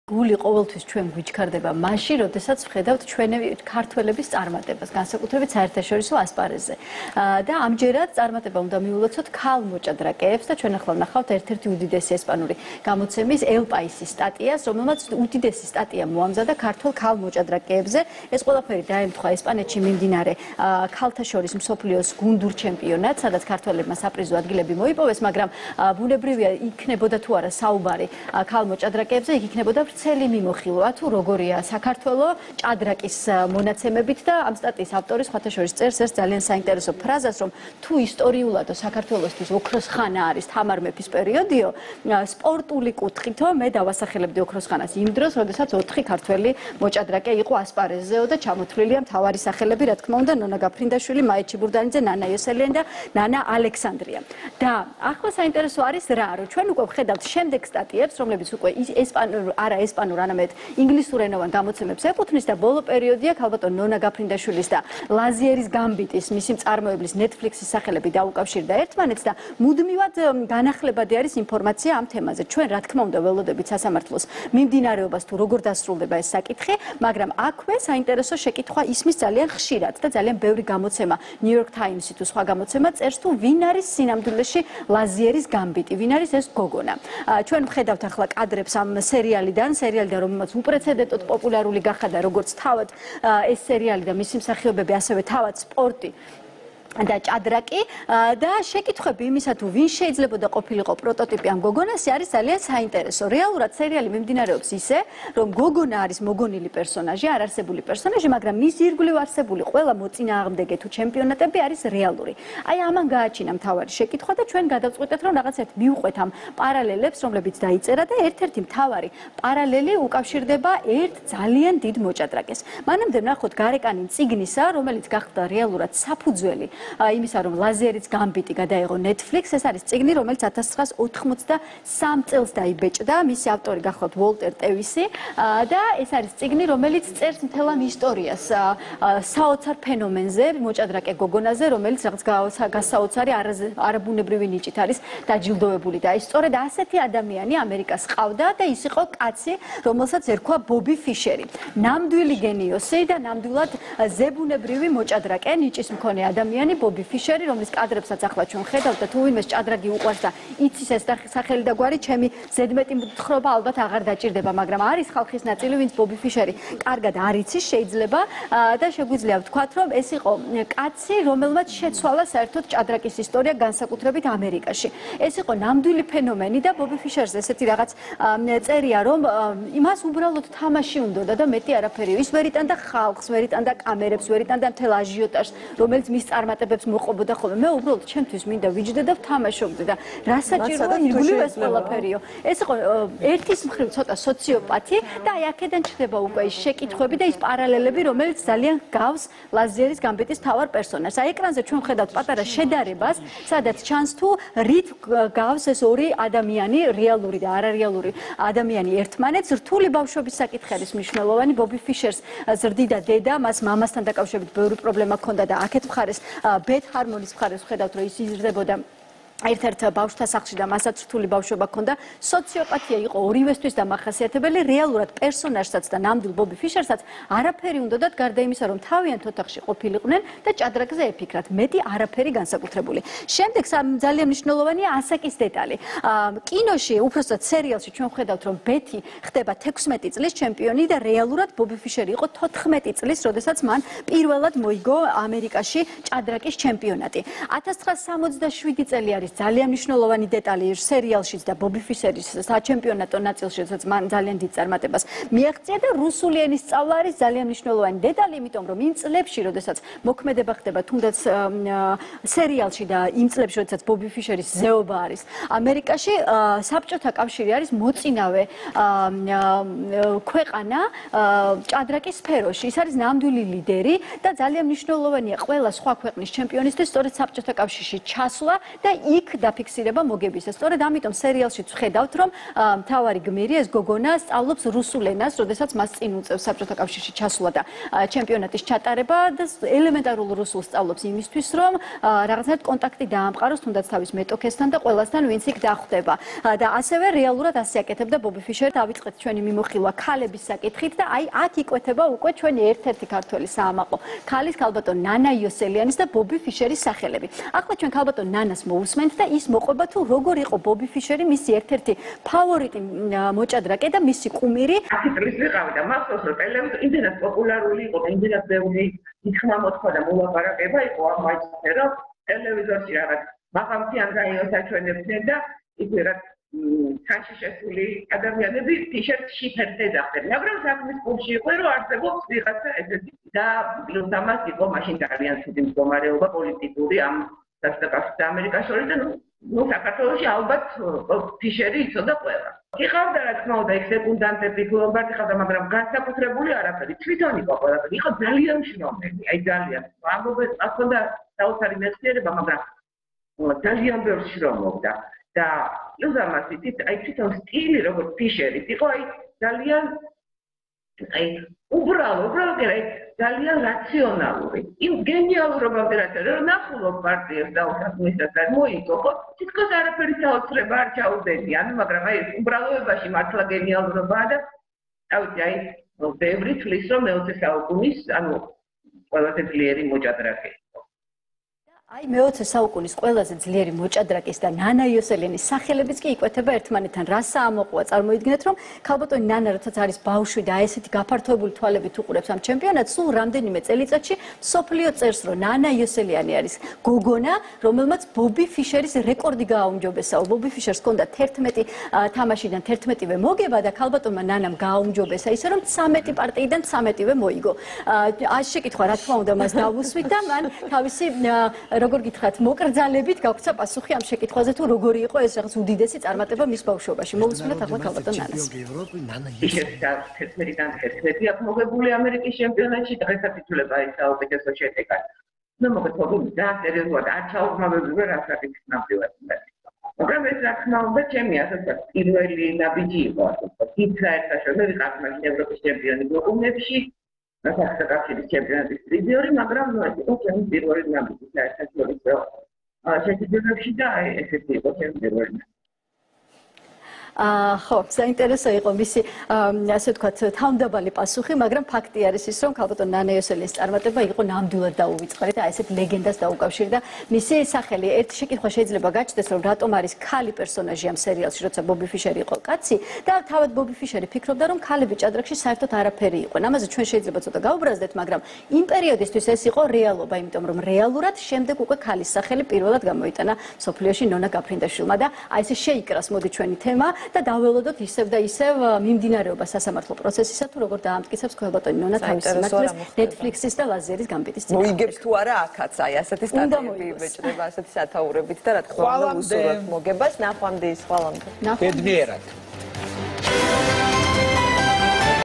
ვინ არის სინამდვილეში "ლაზიერის გამბიტი"?
ქართველი მოჭადრაკე რომელმაც ლეგენდარული ბობინფიშერის რეკორდი გააუმჯობესა!